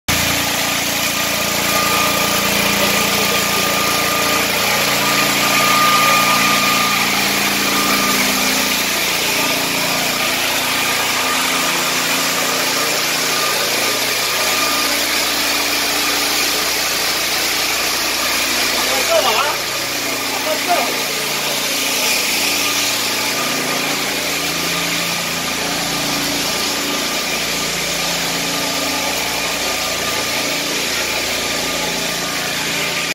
שריפה פרצה בבית הספר מעיין חיים ושלום באשקלון
צוותי כיבוי מתחנת אשקלון פועלים בשריפת ארון חשמל בבית ספר מעיין חיים ושלום באשקלון.